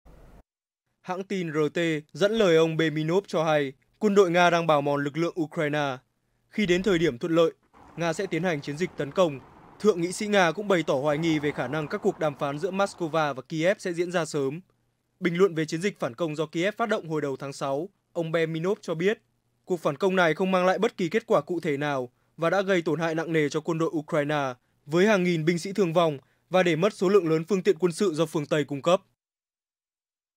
Nga tuyên bố 'bào mòn' Ukraine | Tin tức xung đột Nga - Ukraine Mới Nhất
Thượng nghị sĩ Dmitry Perminov, thành viên Ủy ban Quốc phòng của Hội đồng Liên bang Nga, cho biết quân đội nước này đang "bào mòn" lực lượng của Ukraine, và Nga sẽ tiến hành một cuộc tấn công mới khi đến thời điểm “thuận lợi”.